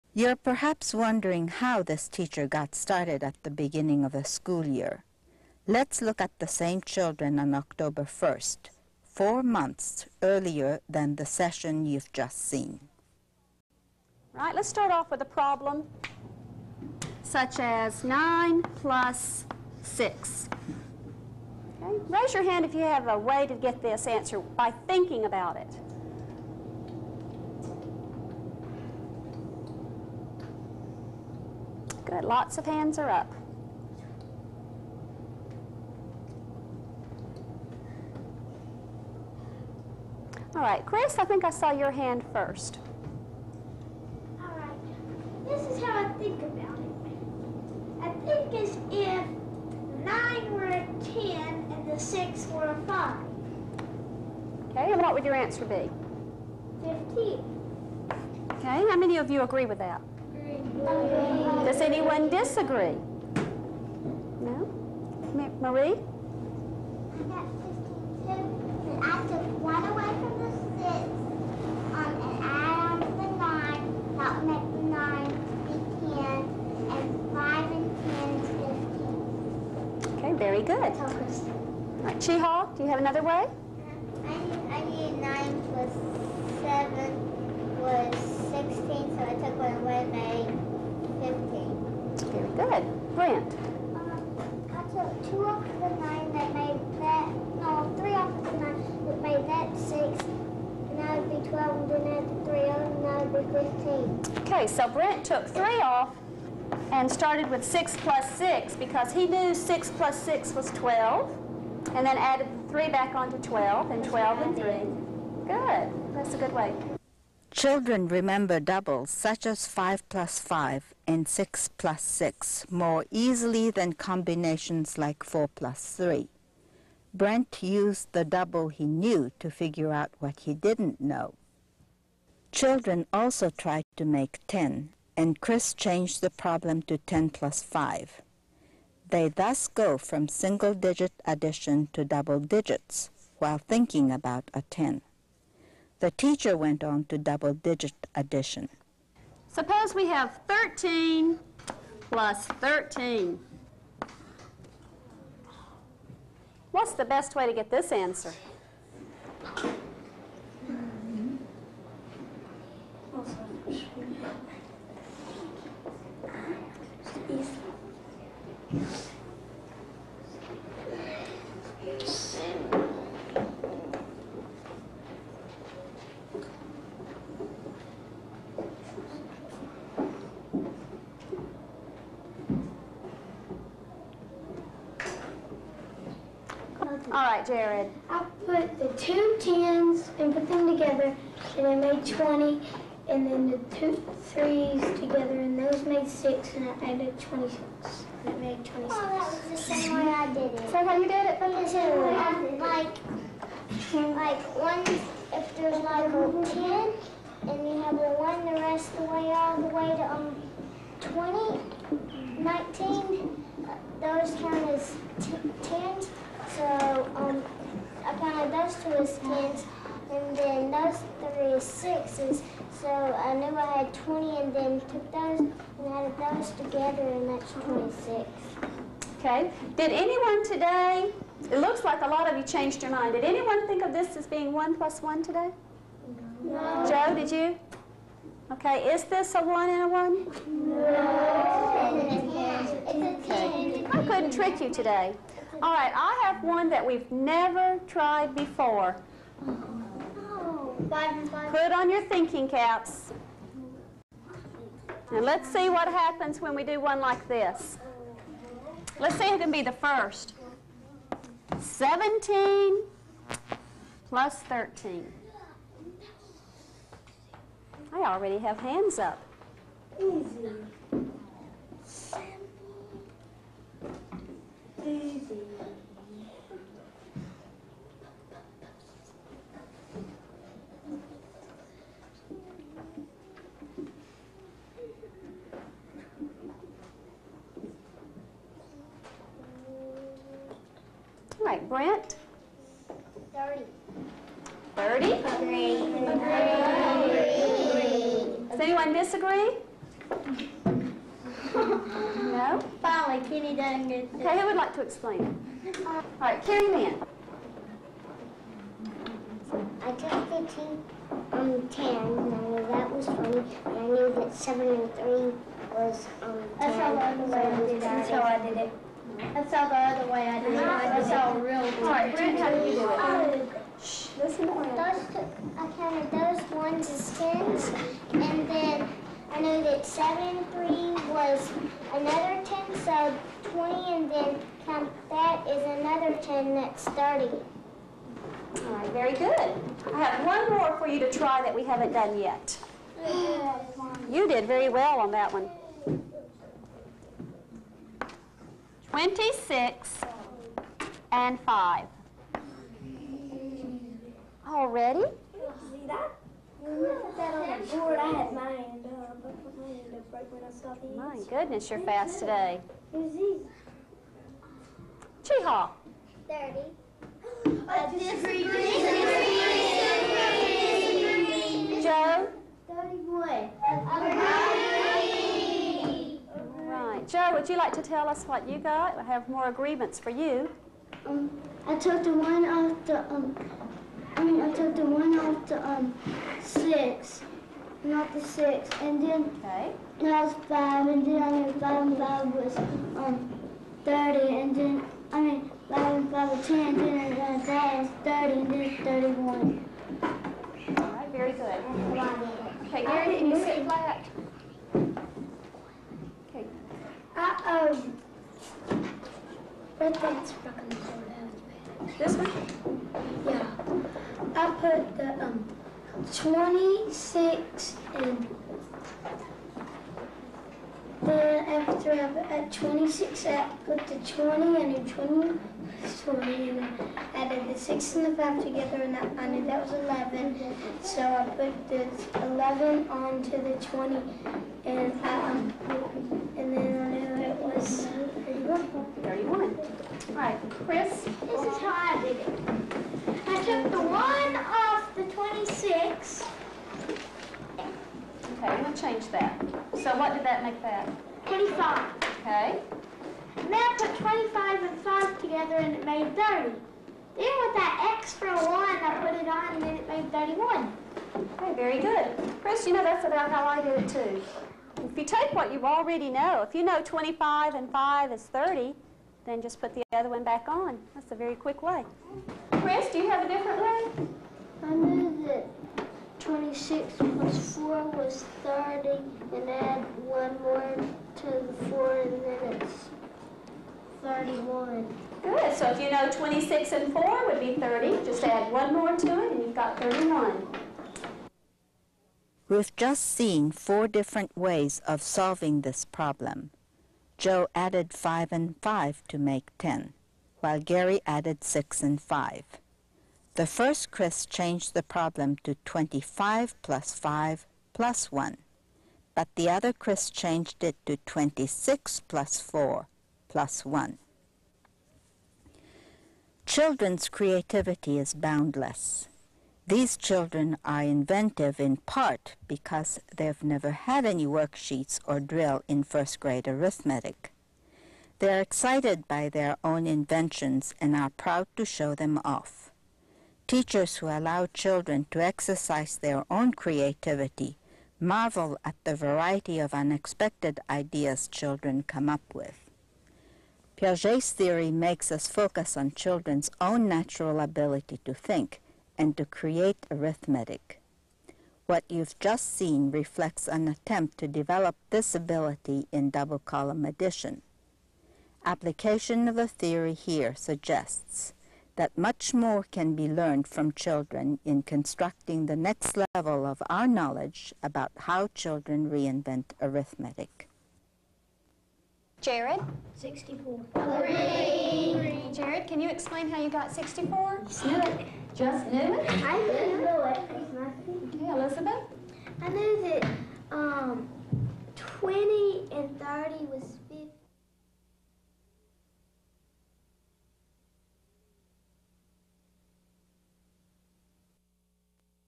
Double Column Addition, Part 3
Kamii, C. (1989). Double-column addition: A teacher uses Piaget's theory (videotape).  New York: Teachers College Press.